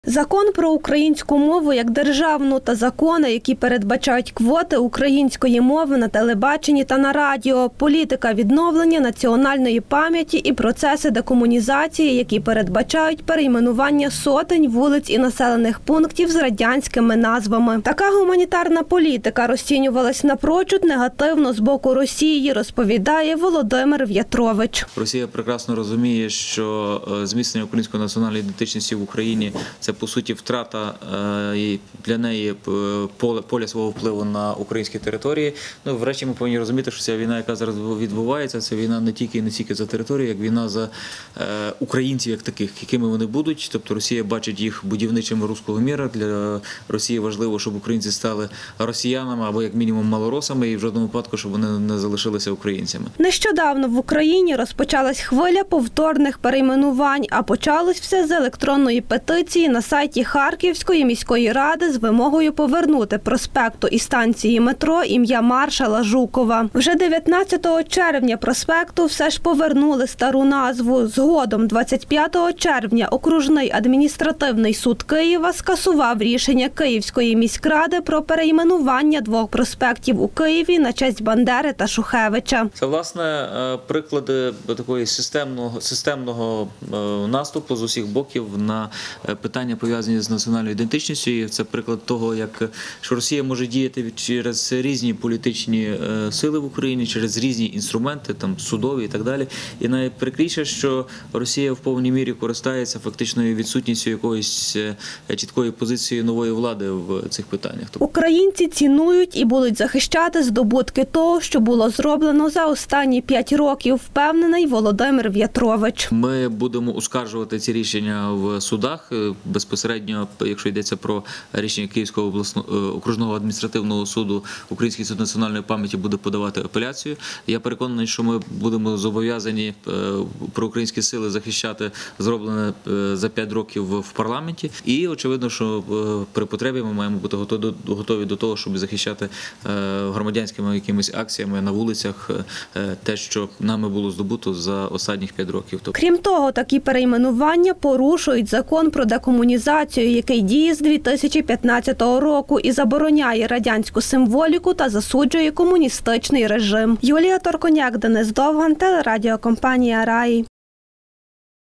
Загроза російського і комуністичного реваншу. Володимир В'ятрович говорив про те, чому це небезпечно
Перейменування вулиць та майданів, демонтаж пам'ятників сумнівним історичним персонам і символіки комуністичного тоталітарного режиму.
Процес декомунізації в Україні як позбавлення від наслідків комуністичної ідеології розпочався в часи розпаду СРСР і стихійно активізувався саме в період Революції Гідності. Проте цьому процесу загрожує небезпека згорання.